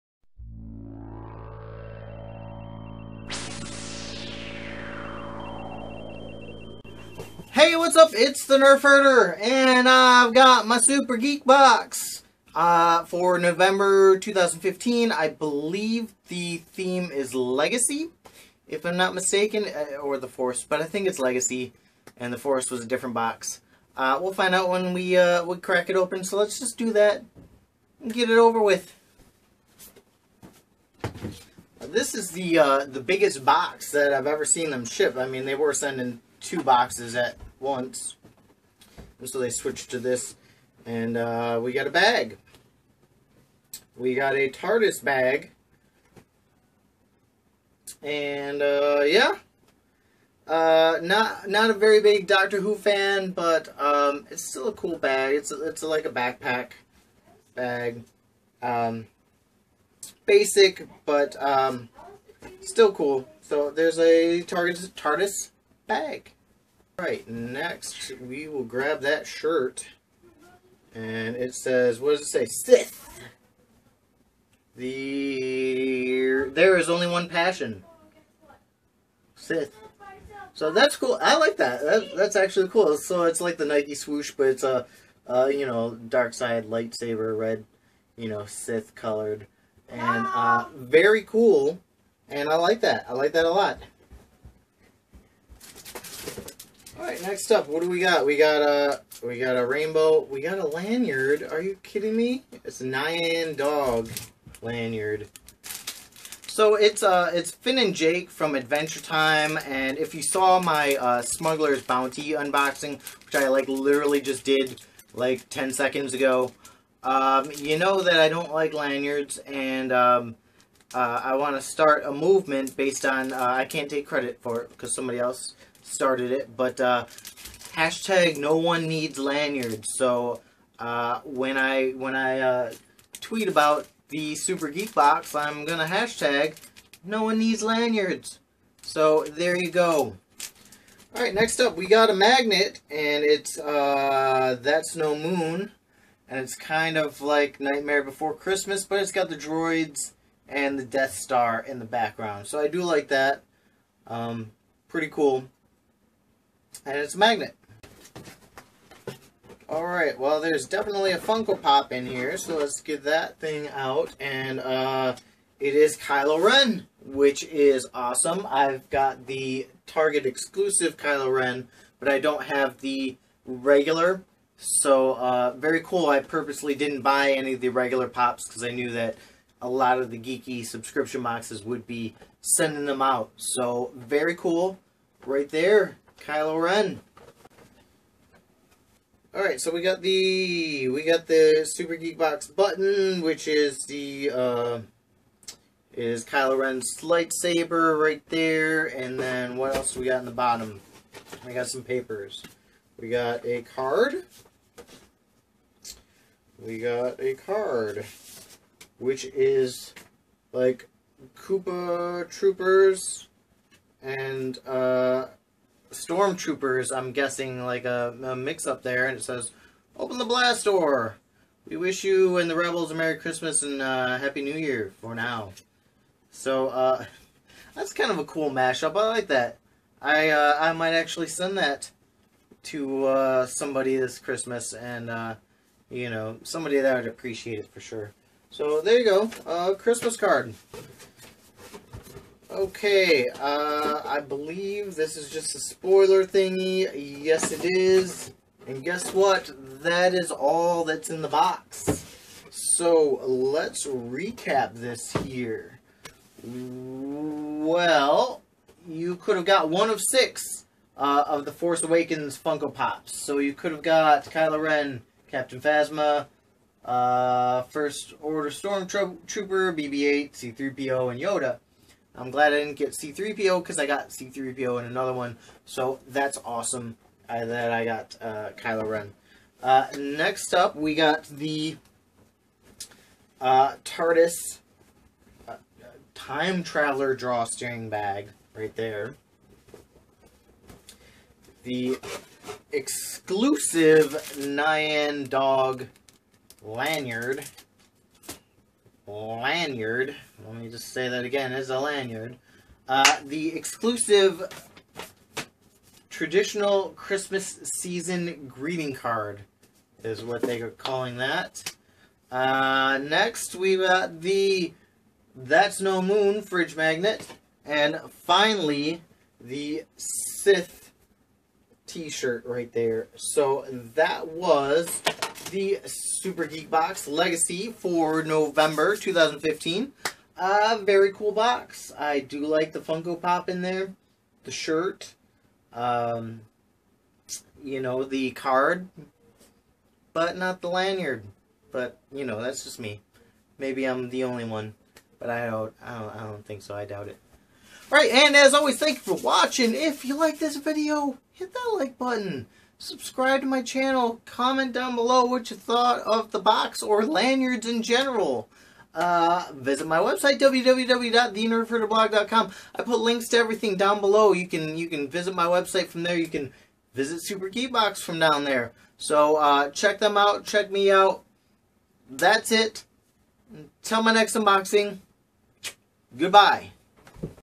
Super Geek Box "Legacy" Unboxing & Review - November 2015
Unboxing the Super Geek Box "Legacy" Box for November 2015